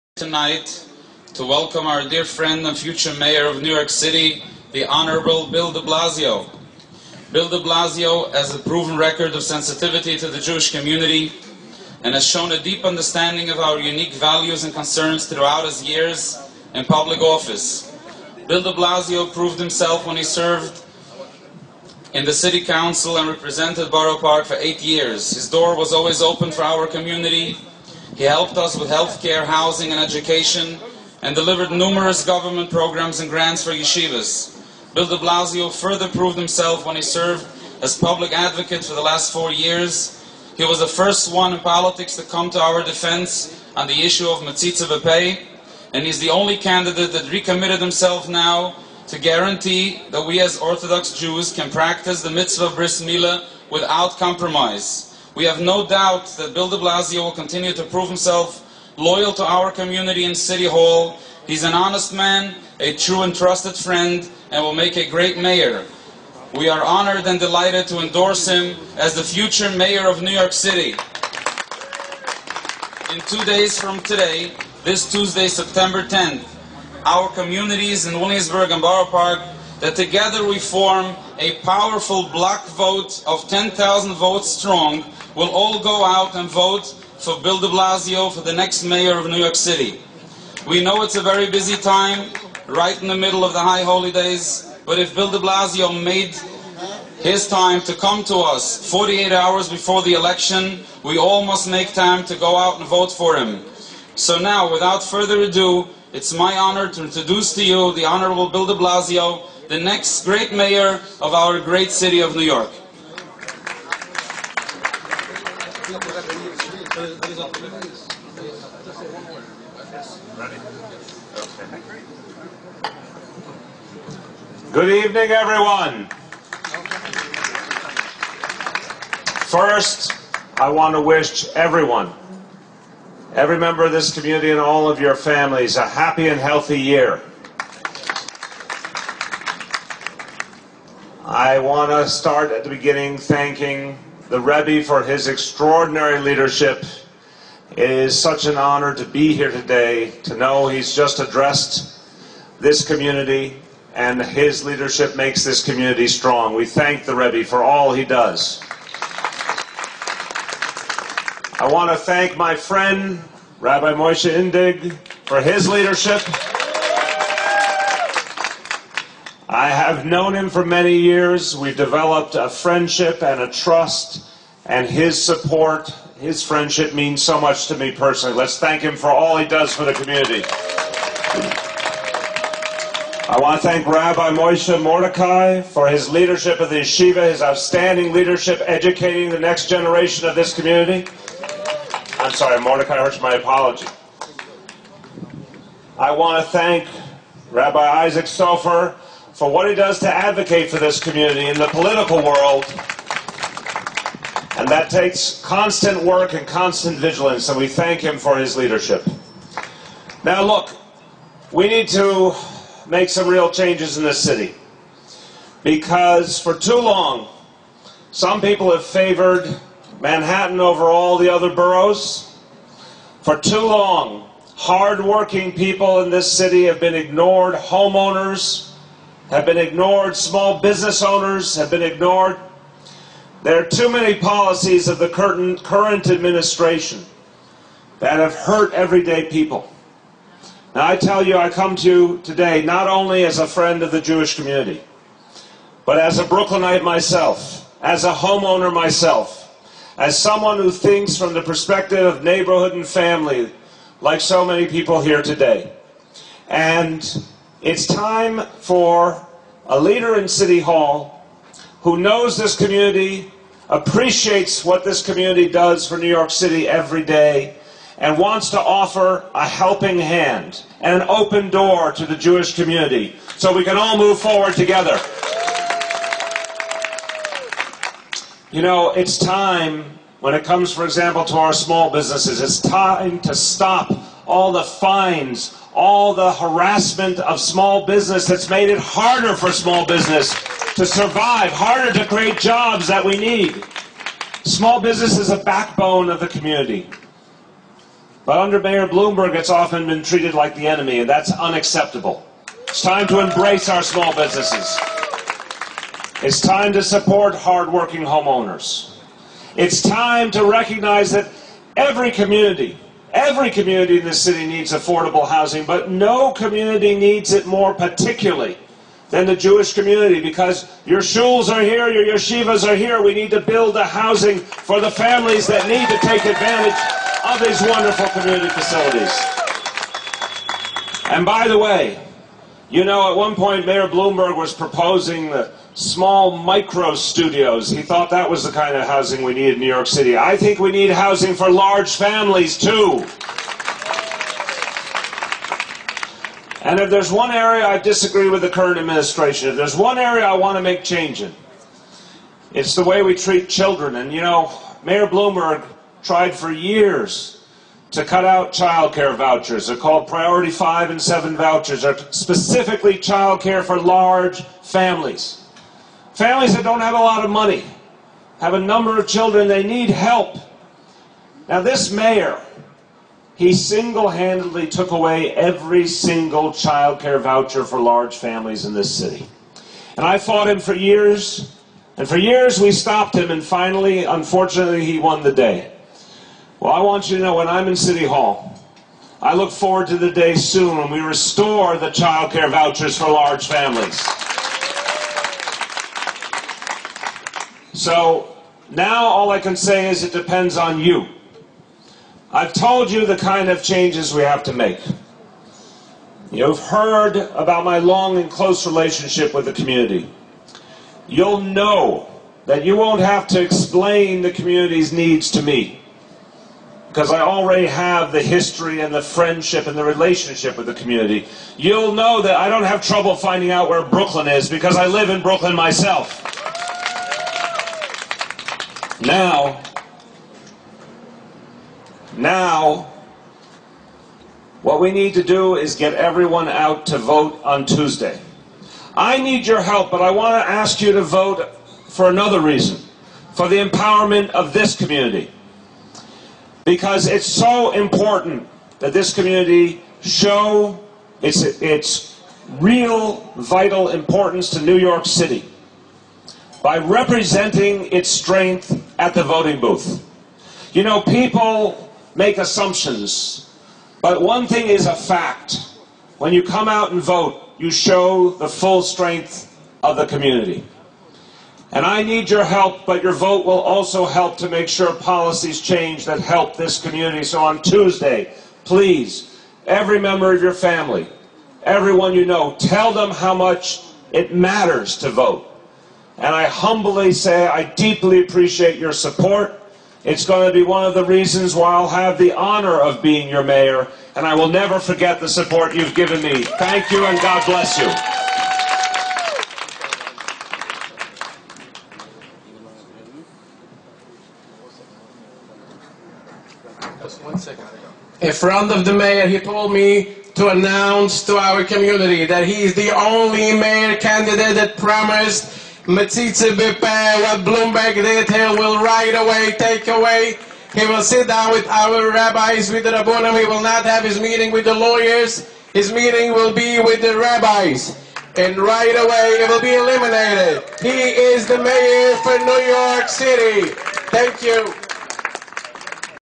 Bill DeBlasio at Satmar Rally Promises to Eliminate Metzitzah B'Peh Ban
Bill DeBlasio at Satmar Aronim Rally in Williamsburg Promises to Eliminate Mayor Bloomberg's Metzitzah B'Peh Ban